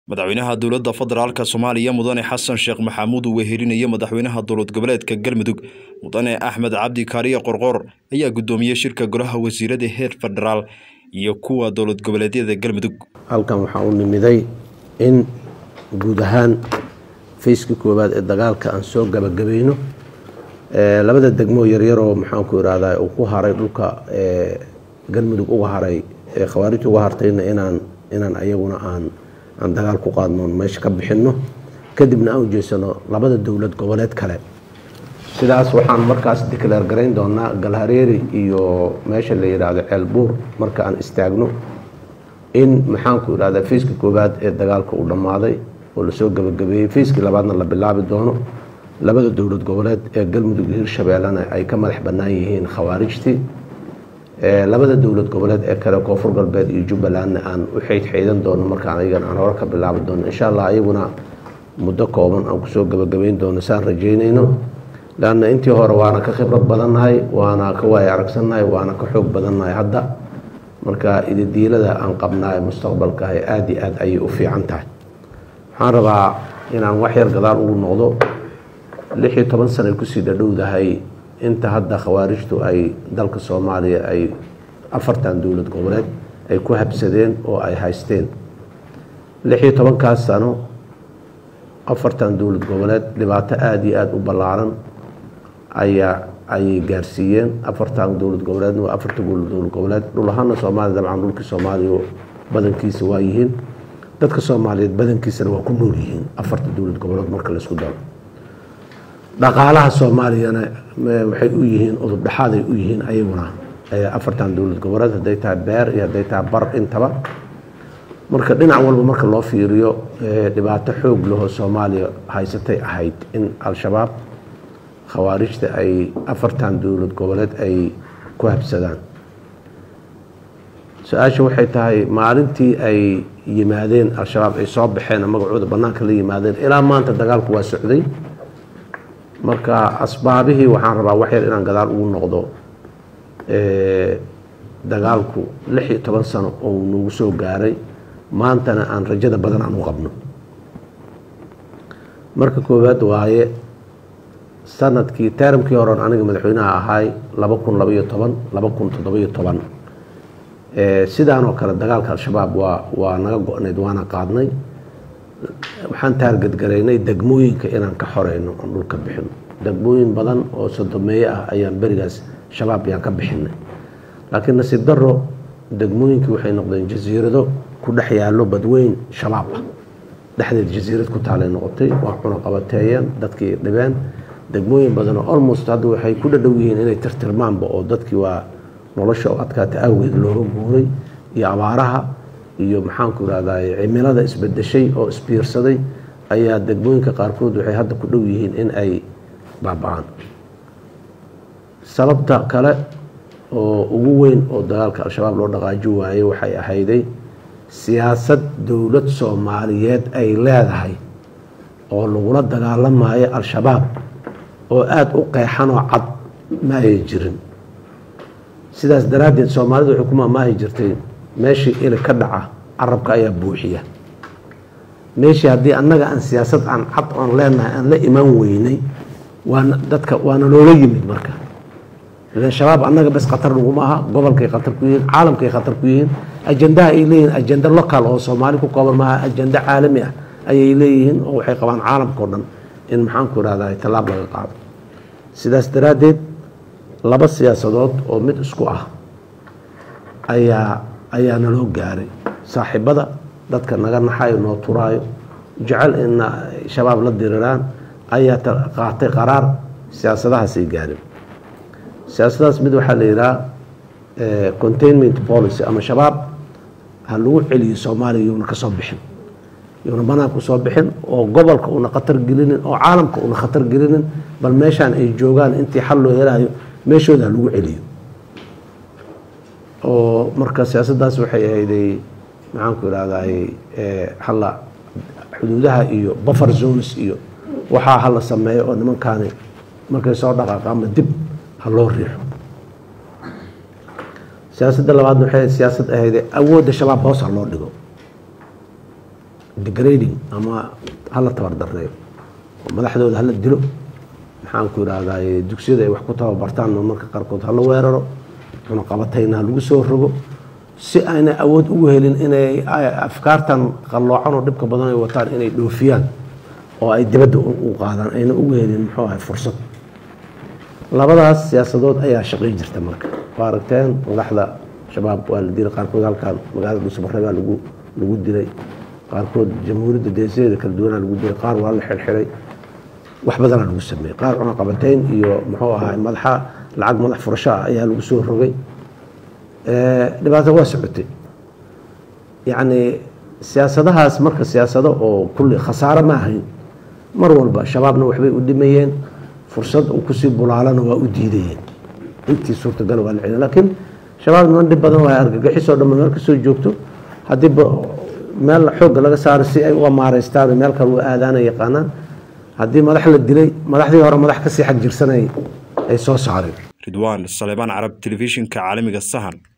Madaxweynaha Soomaaliya Oo Uu Wehlinayo Madaxweynaha Galmudug Ayaa Guddoomiyay Shirka Golaha....
Madaxweynaha Soomaaliya Oo Uu Wehlinayo Madaxweynaha Galmudug Ayaa Guddoomiyay Shirka Golaha Wasiirada Heer Federaal Iyo Kuwa Dowlad-goboleedka Galmudug.